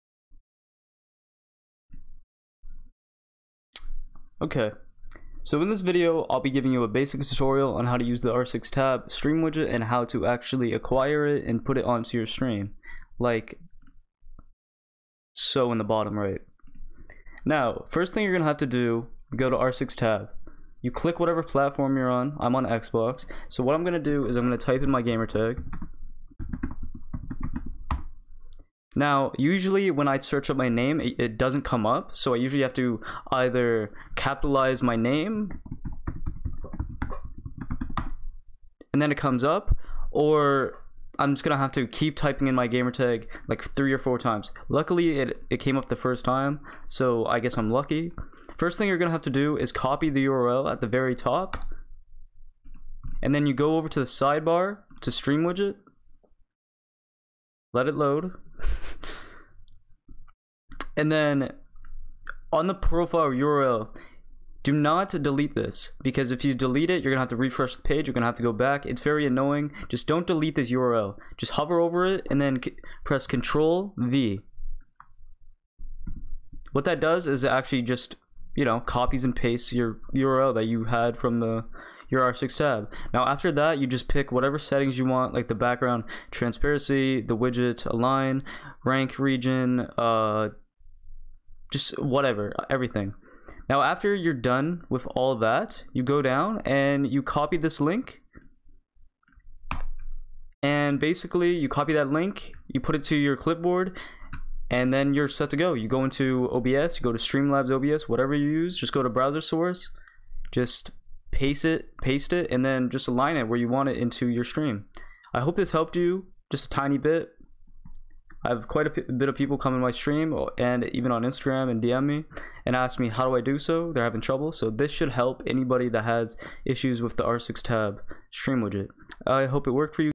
How To Use the R6Tab Stream Widget
In this video I'll give you a basic tutorial on how to use the R6 Tab stream widget.